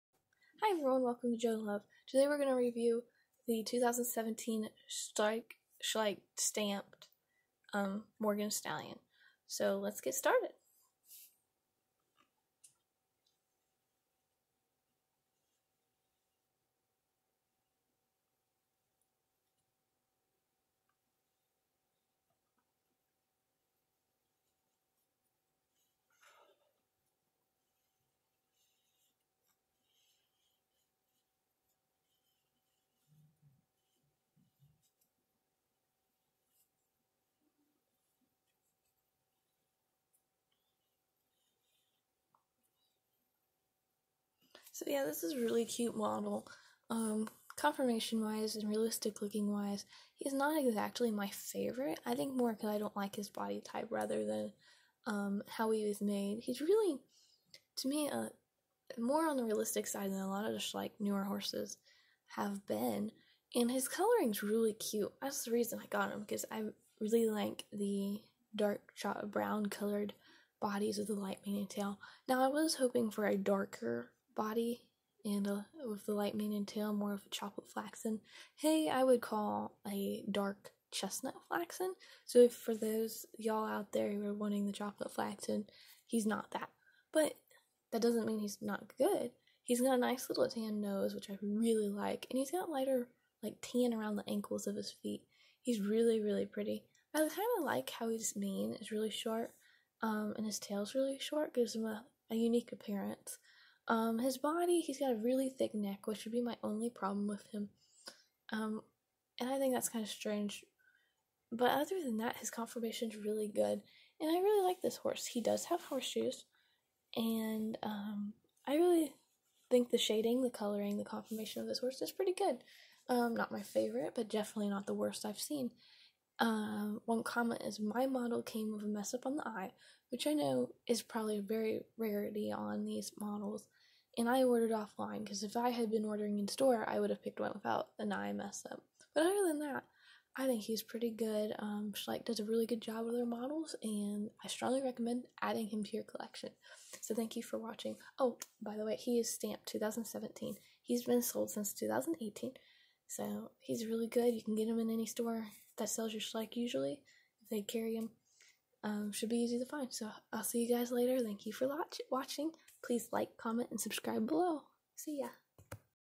Schleich: 2017 Morgan Stallion
Review of the Schleich 2017 stamped Morgan Stallion.